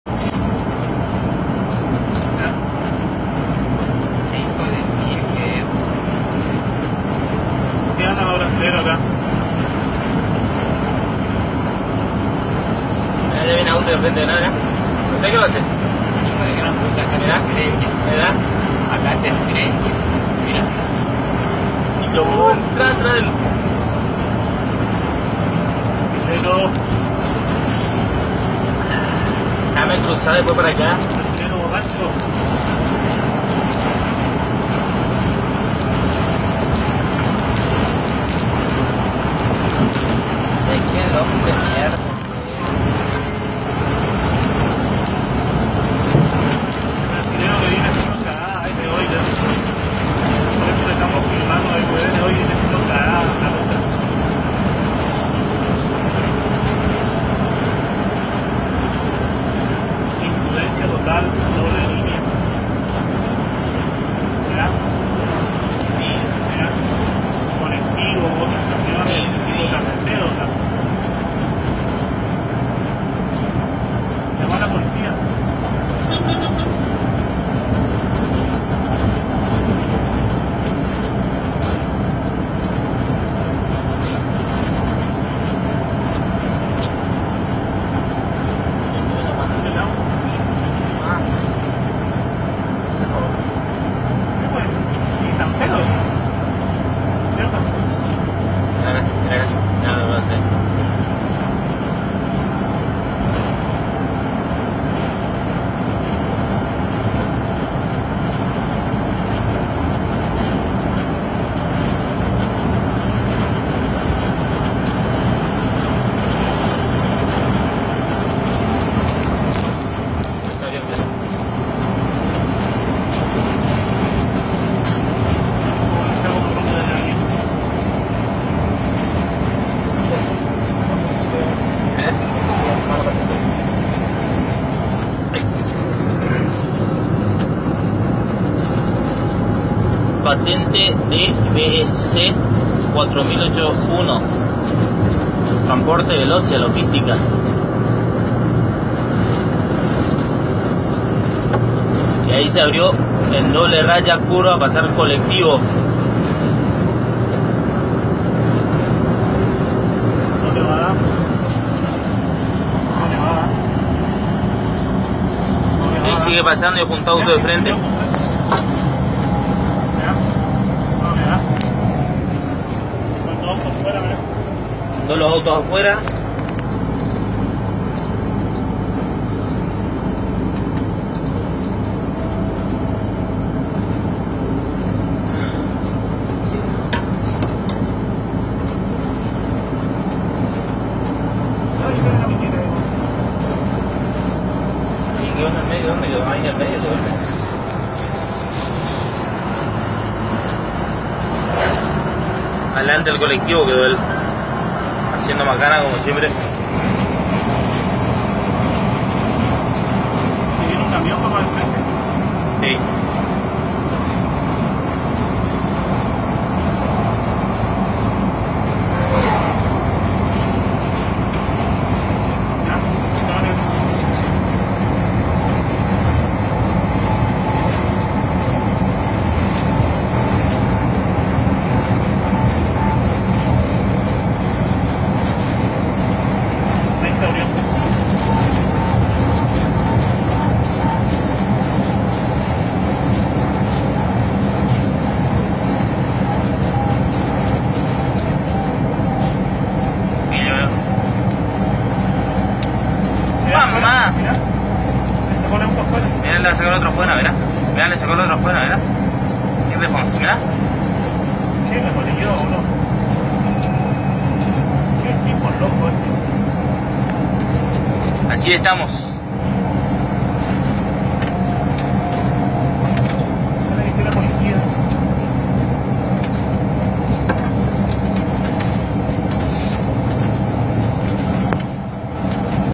VIDEO CAMION IMPRUDENCIA TOTAL
Camionero brasileño causó terror en la ruta 14, lo escracharon tras su paso por Misiones